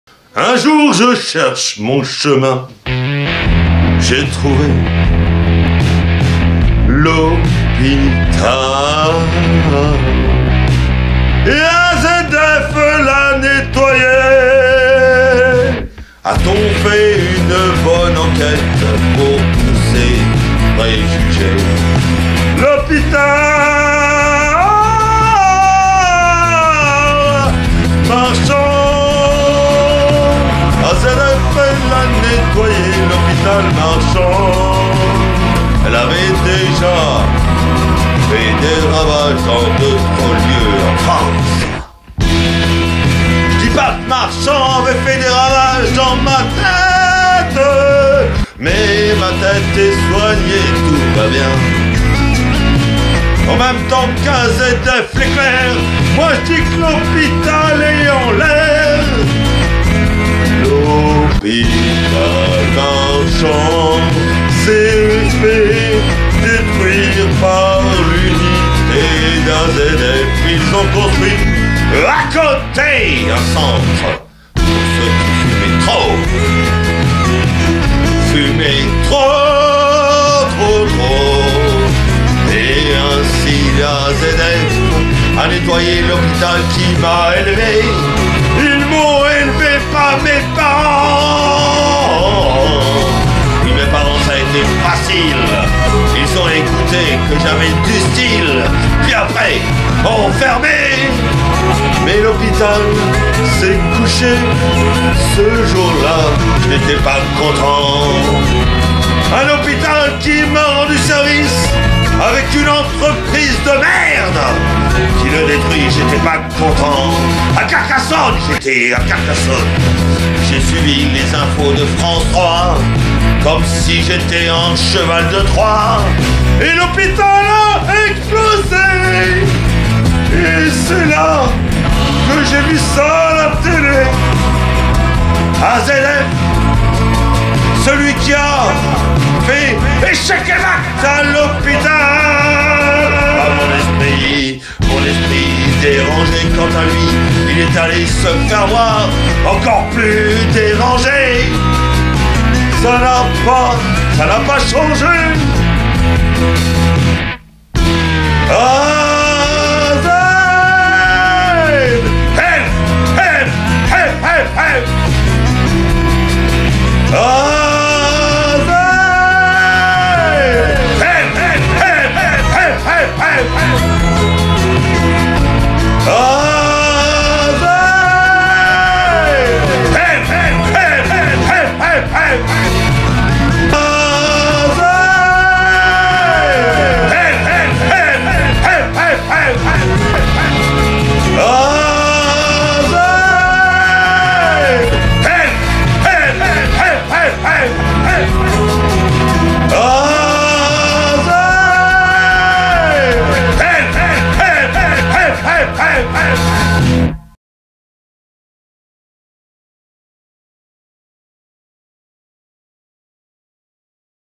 AZF musique défoulement {?...} Quel silence ?
Musique et chant d'un handicapé de l'hôpital Marchant, ce jour-là, sa famille fut réduite en poussière par...